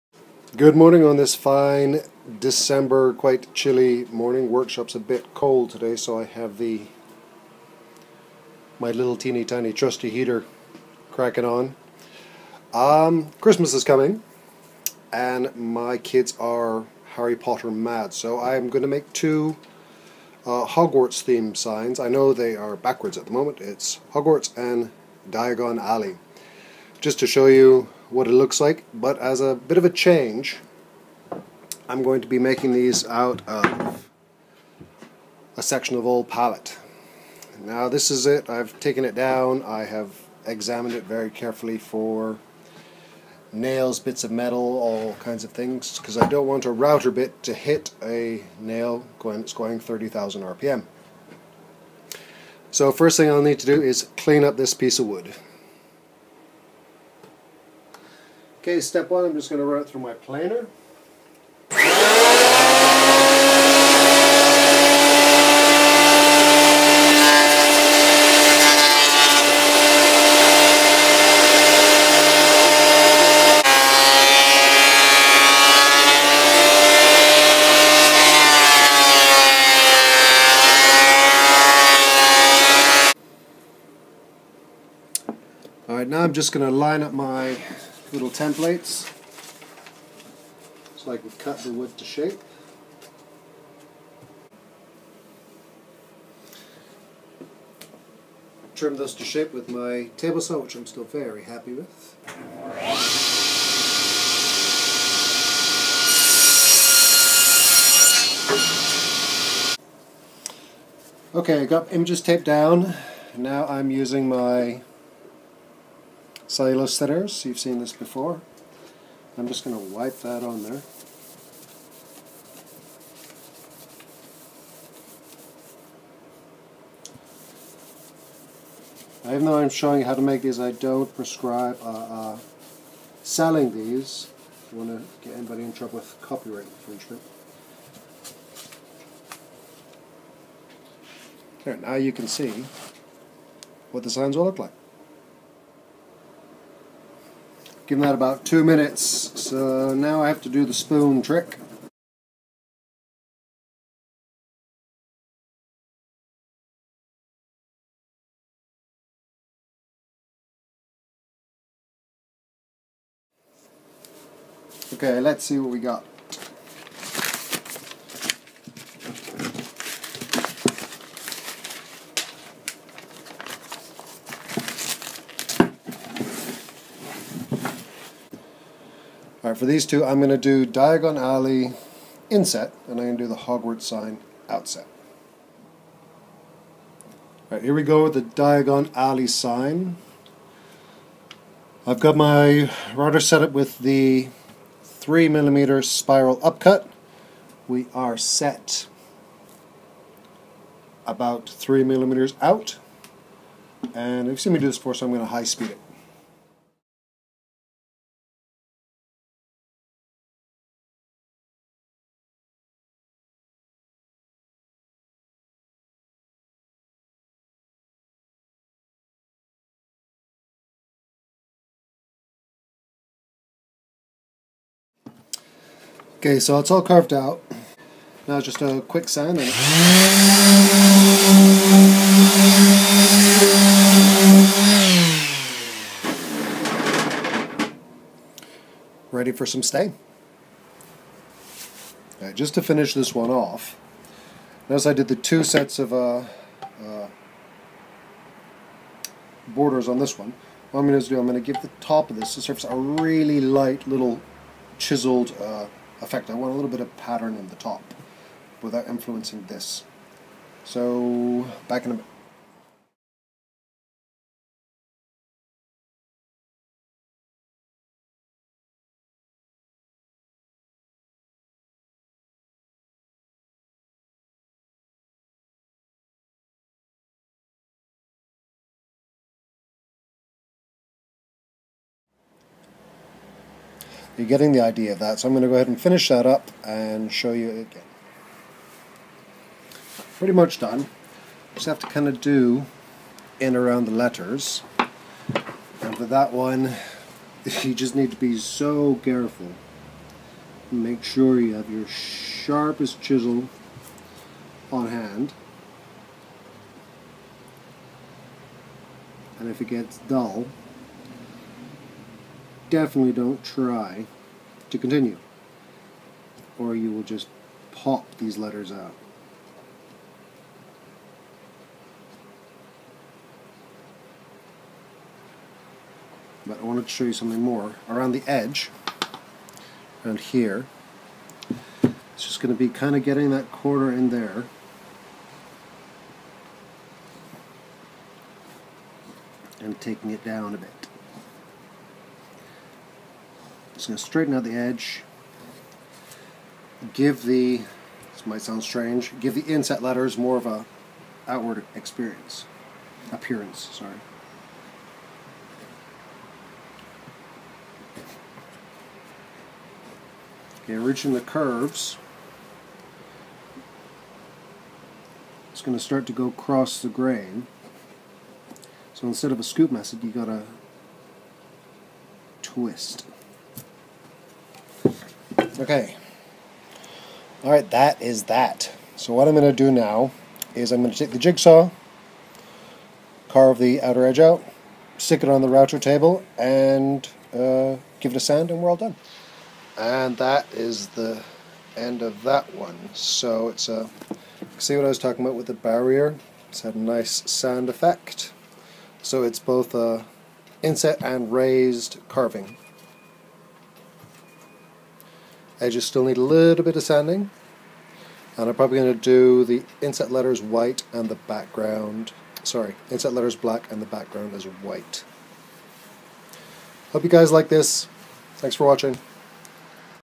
Free hand route a wood Hogwarts sign
In this video I show how I transfer the image to wood and the method I use to route out the sign